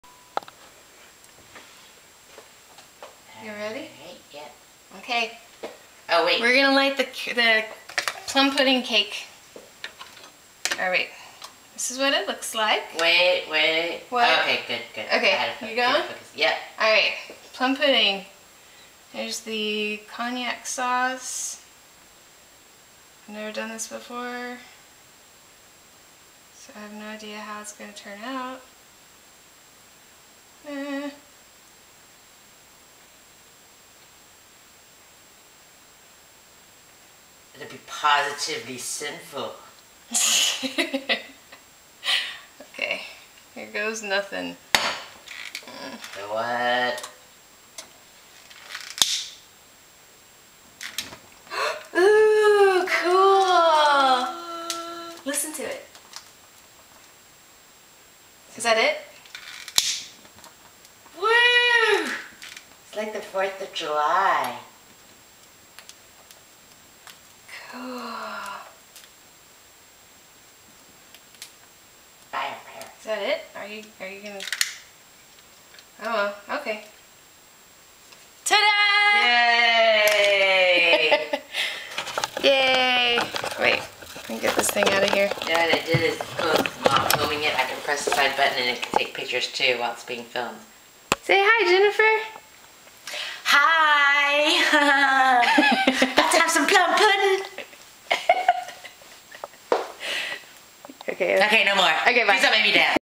Plum Pudding Flambé
My friend Jennifer came to visit, and as fans of Anne of Green Gables, I made a plum pudding for the occasion. I made it from an old recipe that was in a Victorian era magazine called "The Delineator" from 1901. There were a couple variations made, but not many. I kept it as authentic as possible using only what ingredients would have been available back 111 years ago. The final product turned out pretty tasty. (The Louisiana Hot Sauce just happened to be sitting there. It was not used in the pudding.)